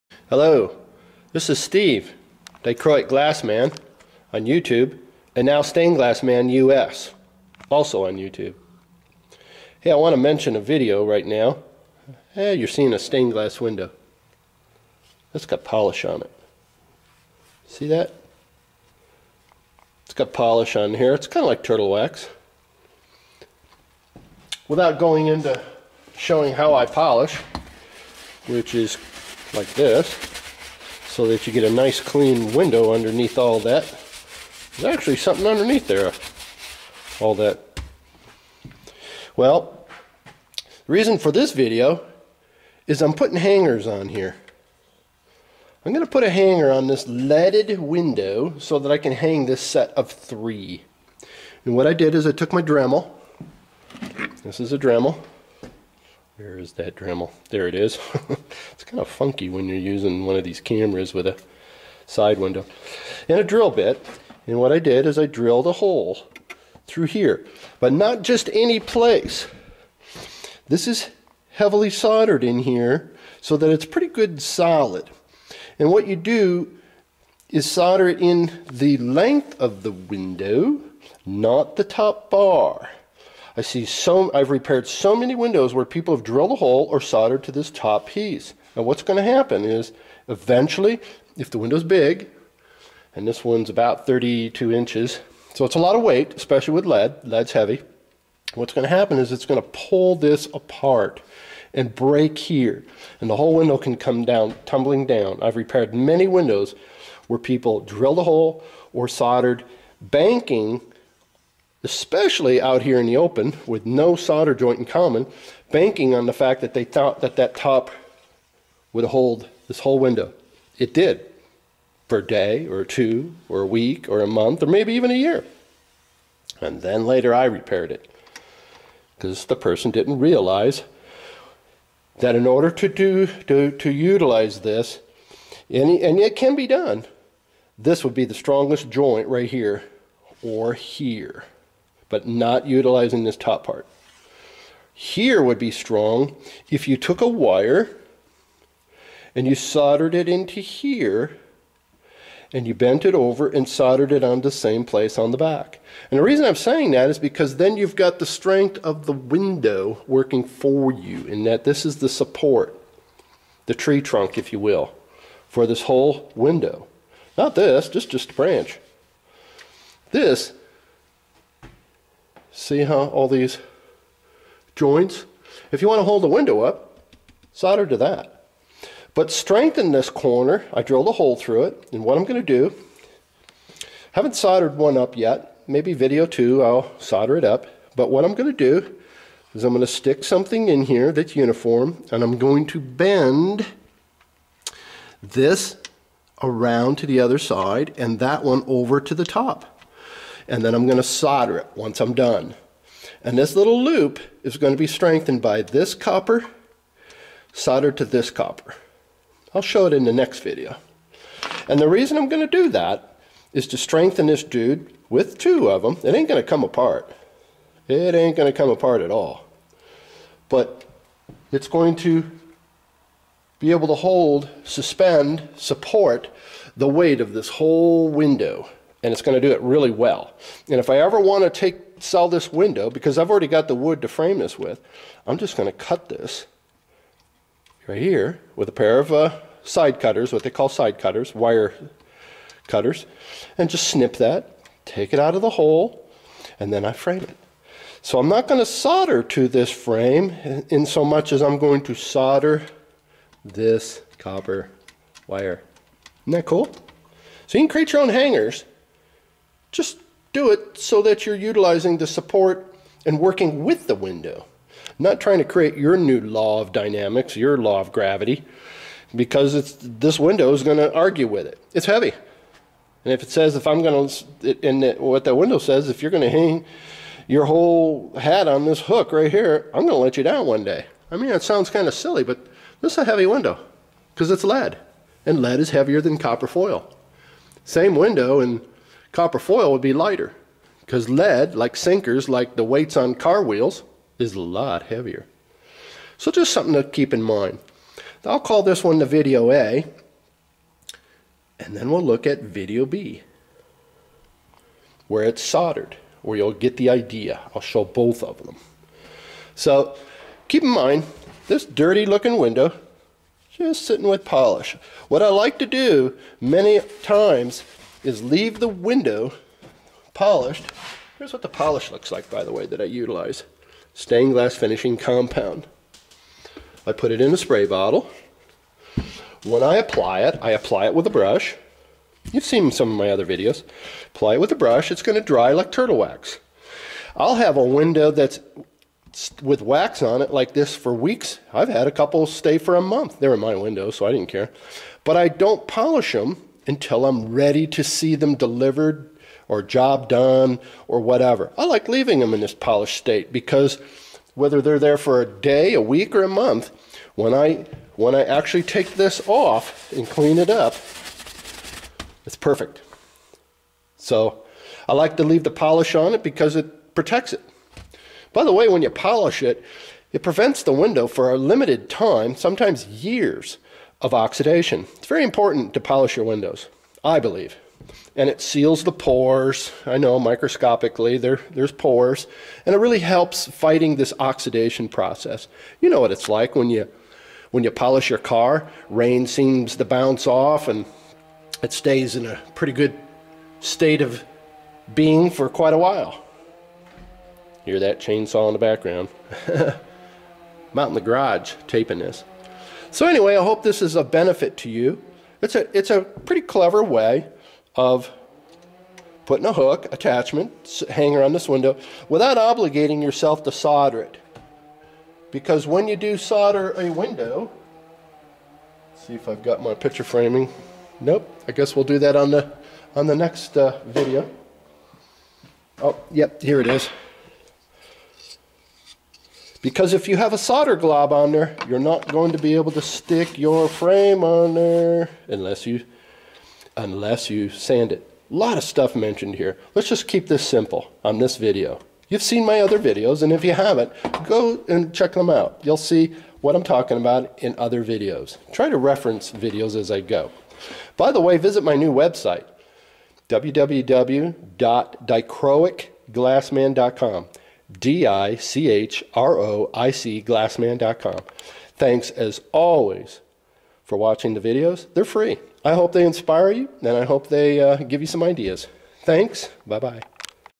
stained glass how to ideas sg17a window hangers
created a way of hanging a fairly heavy stained glass panel using a heavy grade of copper like a coat hanger and how to attach it to the panel